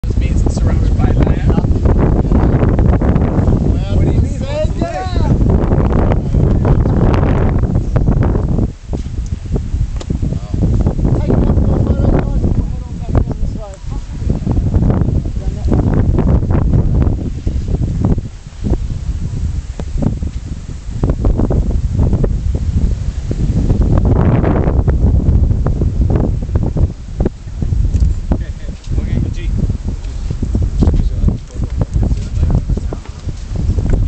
WHITSUNDAY ISLAND, CAIRNS, AUSTRALIA
BARRIER REEF, SPECTACULAR VIEW FROM TOP OF THE ISLAND